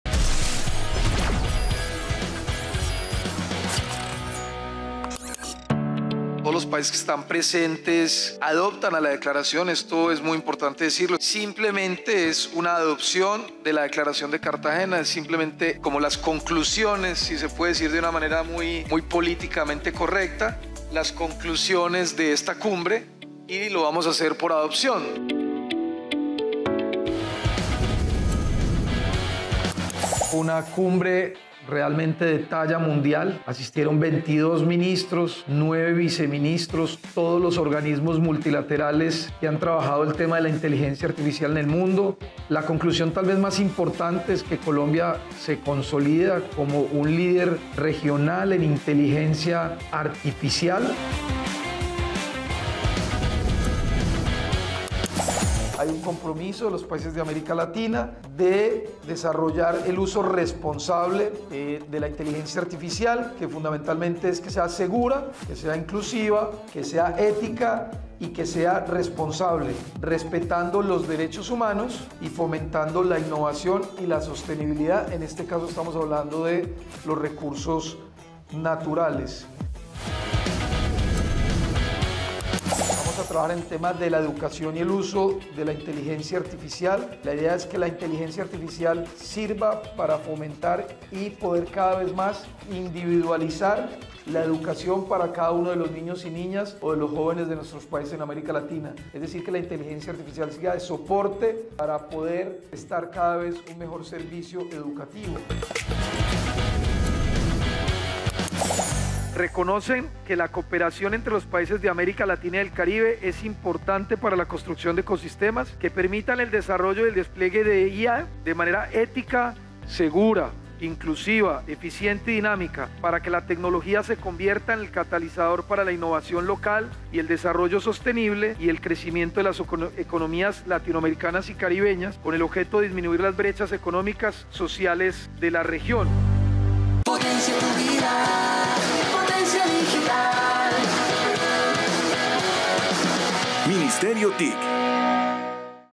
Colombia se consolida como líder regional en IA
El compromiso alcanzado para promover marcos de Gobernanza para la IA, para el desarrollo  y uso seguro, inclusivo, ético y responsable de esta tecnología, respetando los derechos humanos en la región. A demás frente a la educación, la declaración de Cartagena, manifiesta la decisión de los países de fomentar la formación y capacitación en temas digitales, así como intercambio de buenas prácticas respecto al uso de la IA en el sistema educativo.

Los países reconocen la importancia de la cooperación para la construcción de Ecosistemas que permitan el desarrollo y despliegue de la IA para que esta tecnología se convierta en catalizador de la innovación local, el desarrollo sostenible y el crecimiento de las economías latinoamericanas y caribeñas.